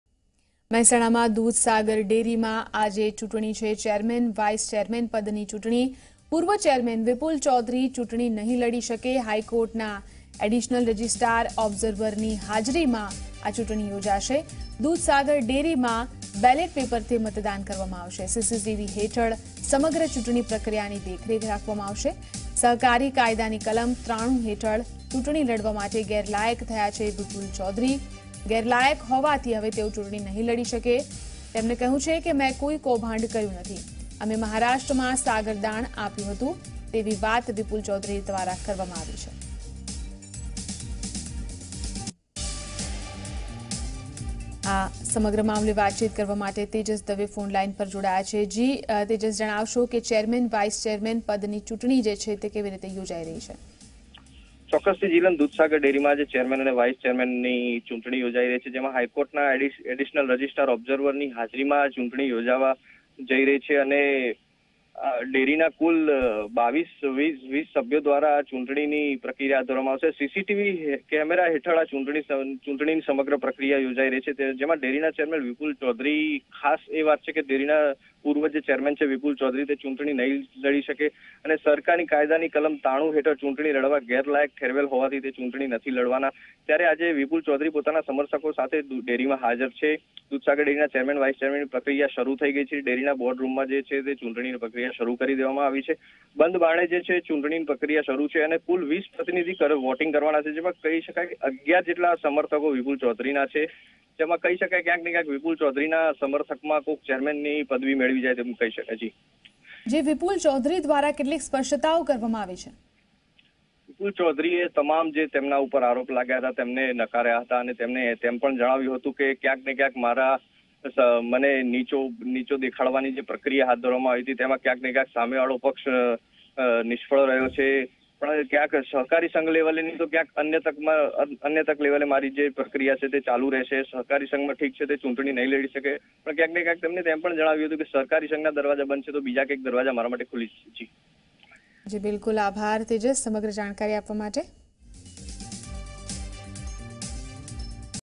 Mehsana: Chairman, vice chairman election in Dudhsagar dairy - Zee 24 Kalak
Mehsana: Chairman, vice chairman election in Dudhsagar dairy